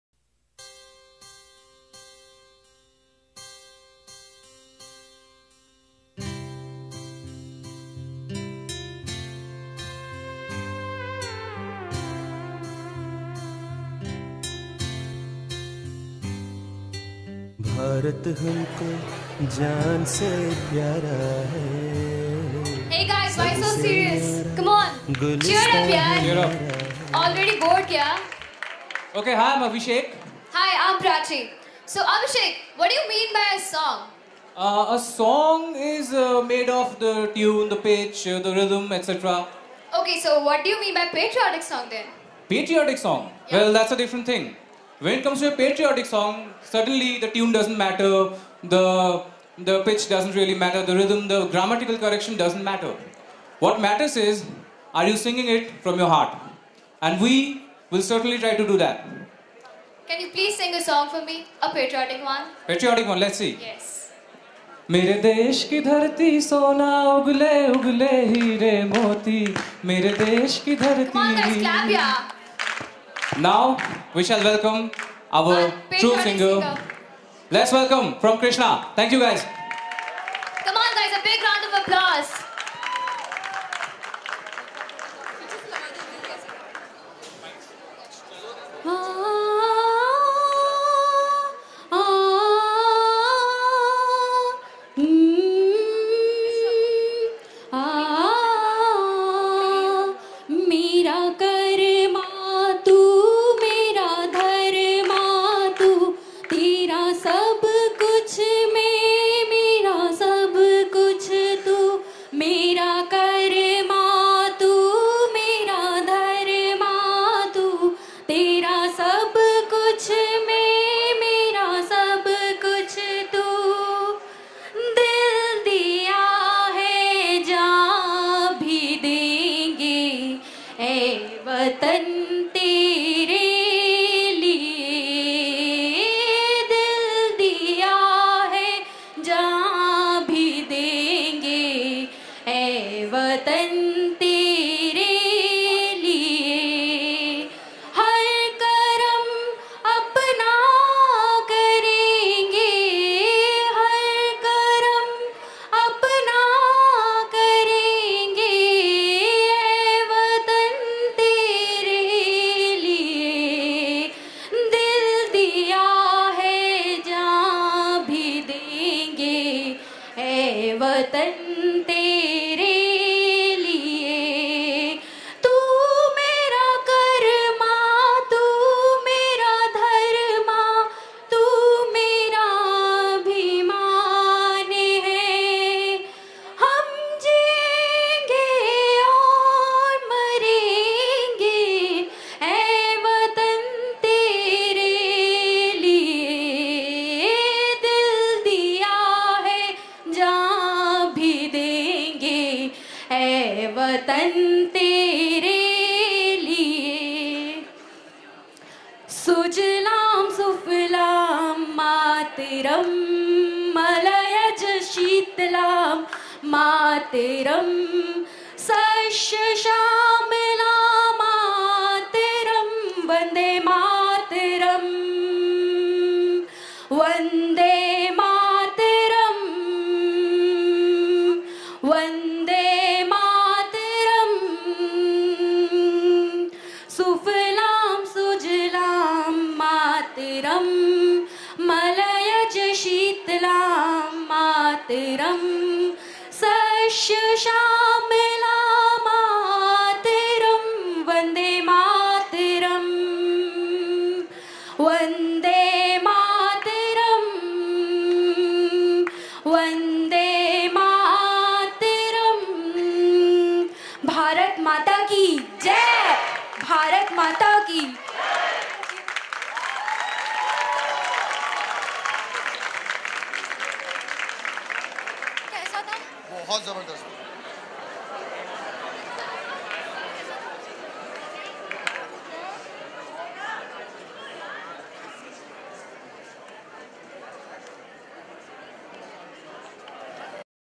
Patriotic Singing River - KRISHNA Aiyaswamy Cultural Competition 2016 @ Sri Balaji Society, Pune
Students Manager of KRISHNA River performing Patriotic Singing in Aiyaswamy Cultural Competition 2016 held at Sri Balaji Society, Pune

Edited By : -
SHIVAM GUPTA
AVINASH ANSHU
APURBA PRASAD
DIVYANSH SINGH